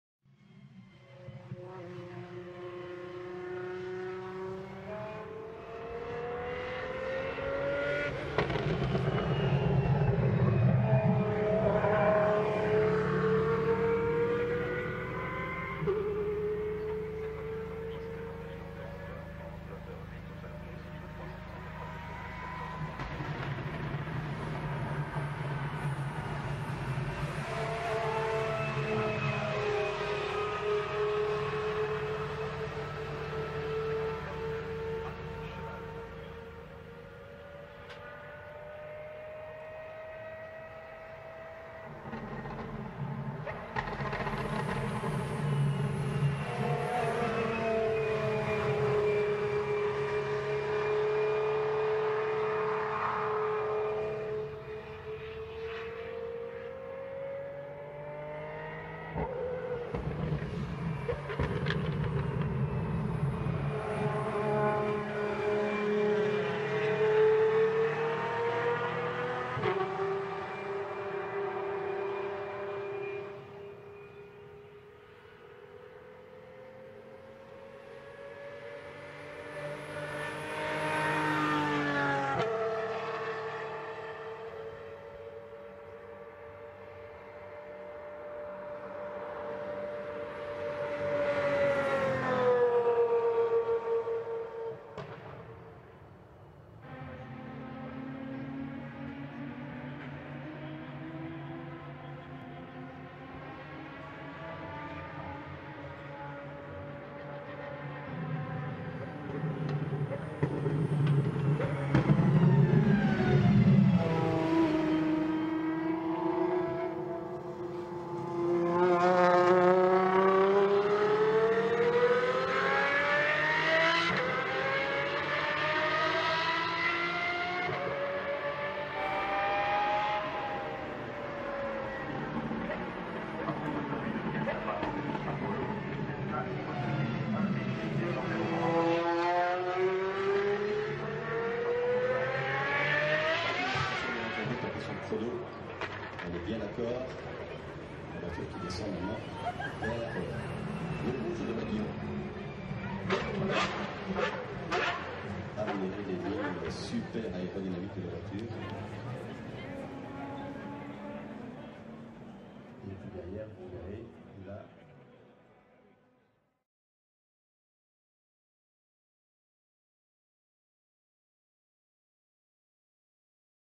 Apollo IE 'Red Dragon' driving on a rainy Spa Francorchamps
In this video you wil see and hear the Apollo IE 'Red Dragon' driving around on a rainy Spa Francorchamps

Please support my carspotting buddy's